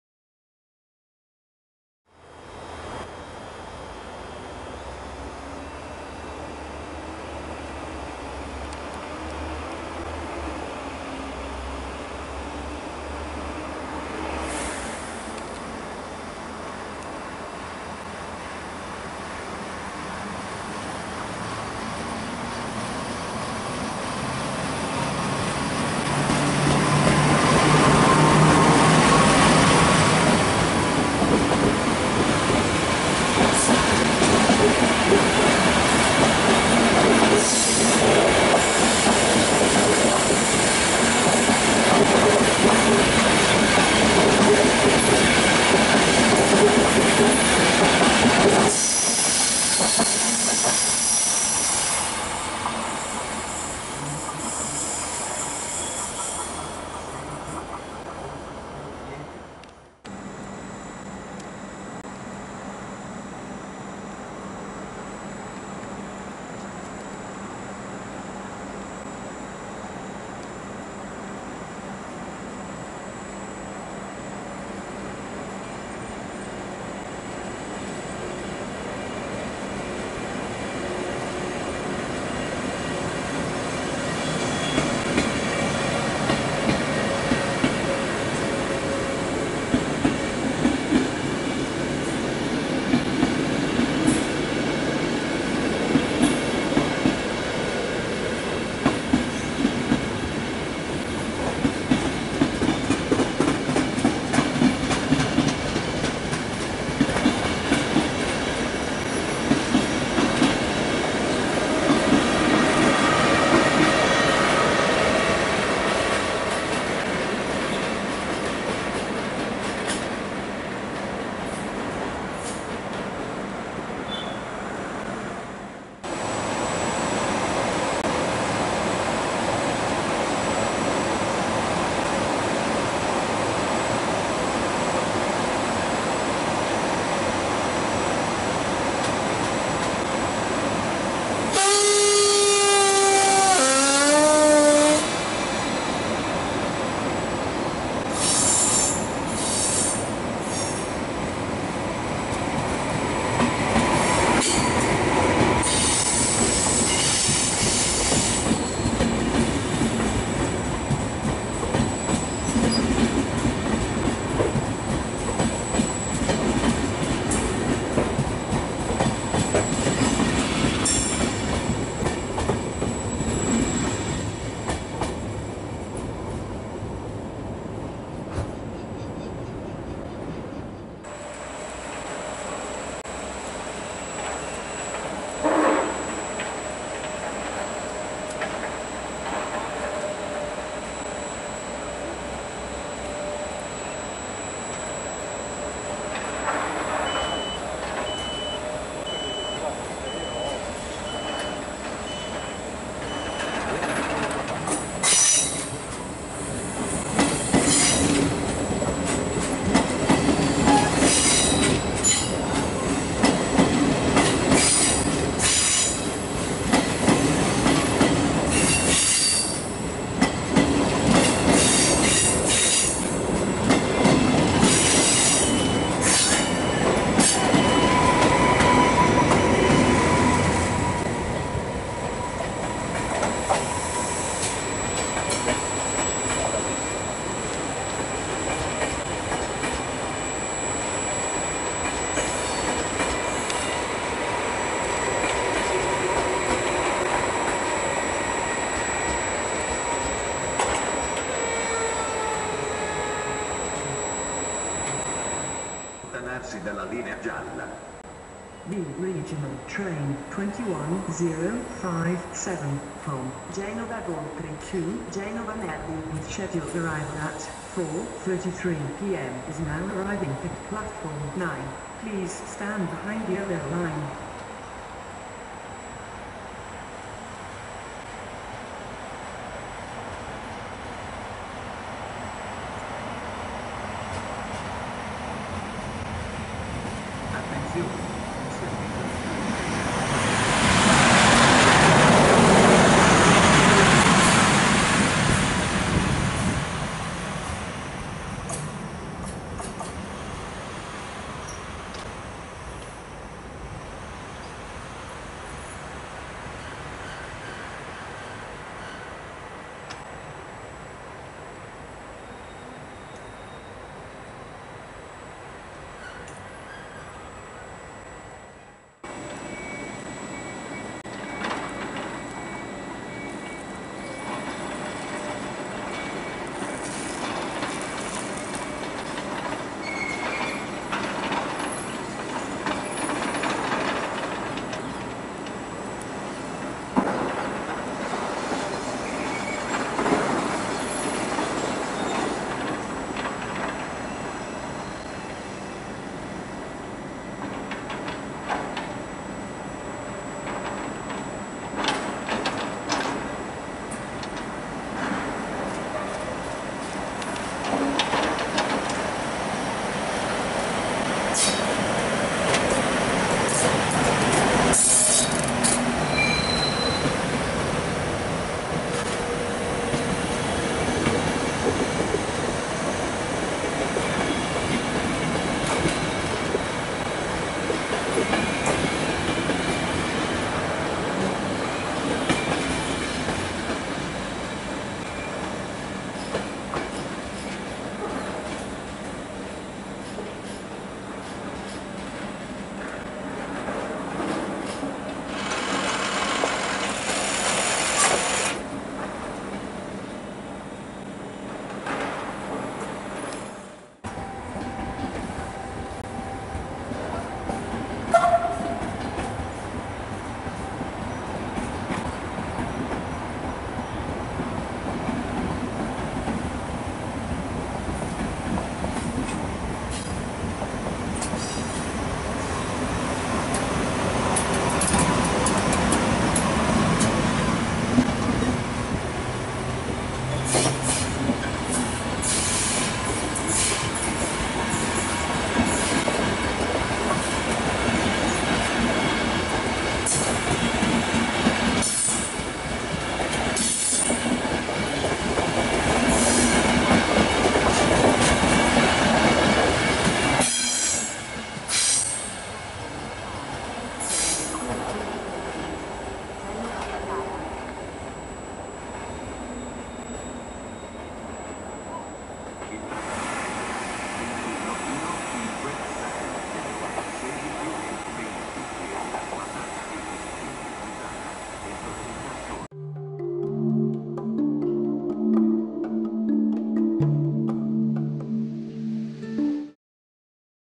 A tutto treno
Carrellata di arrivi e partente dalla stazione di Genova Brignole in una fredda giornata di Gennaio